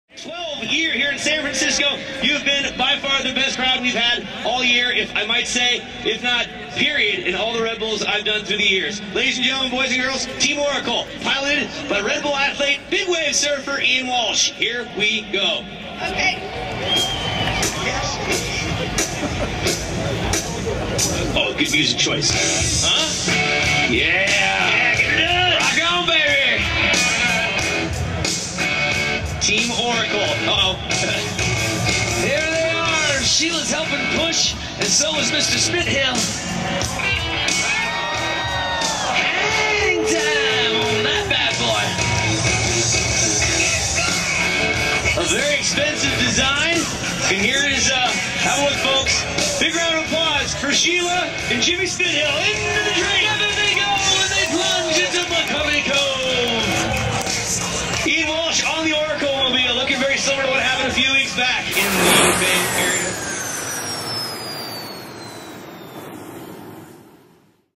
Ian Walsh and Oracle - Red Bull Flutag 2012 San Francisco
Red Bull Flutag 2012 San Francisco